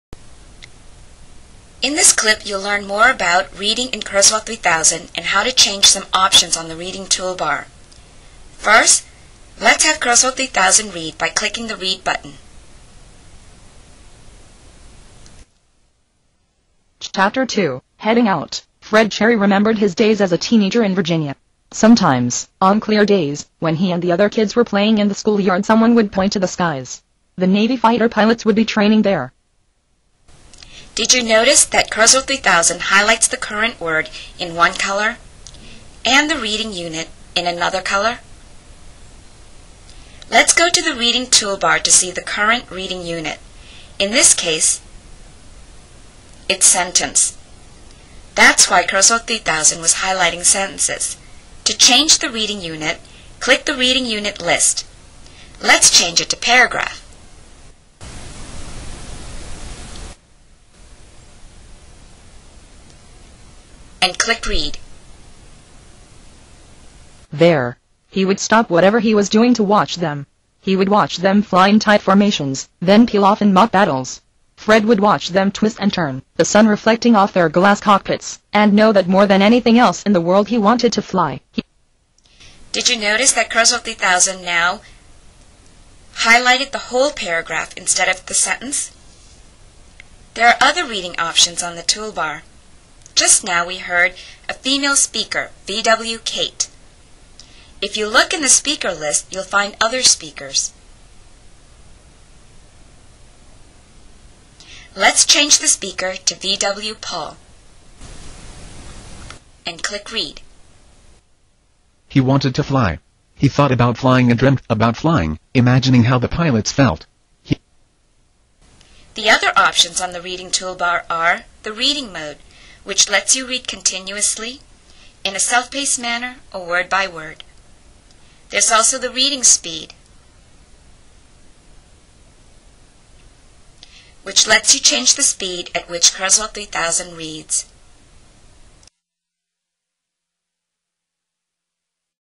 Kurzweil 3000: Change Reading Options Video
In this video you'll learn about reading in Kurzweil 3000 and how to change options in the Reading Toolbar.